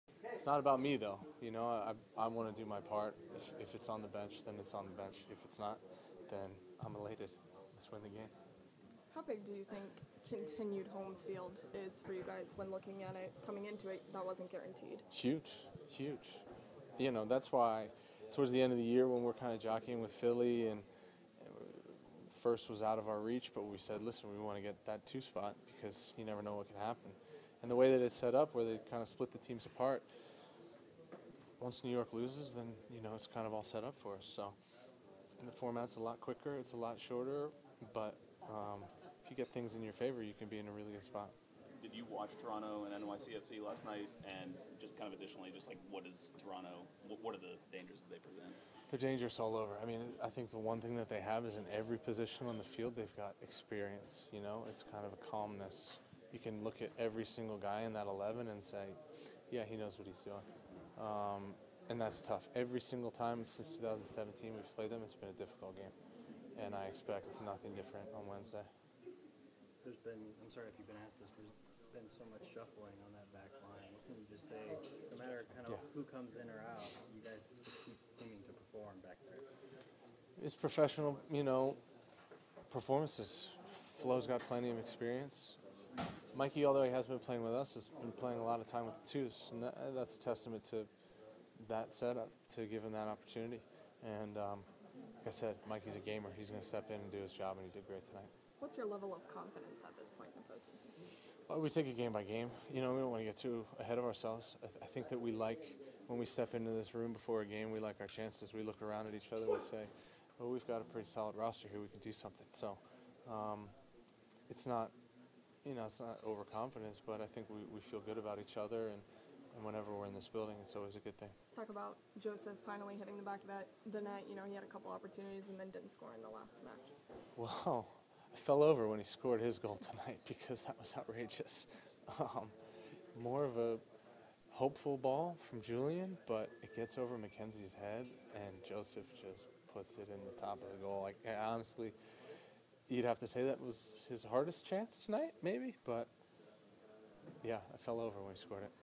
Atlanta United's Jeff Larentowicz on next game against Toronto: They know what they're doing
Jeff Larentowicz said the next game may be a tough one, but they plan to give it their all.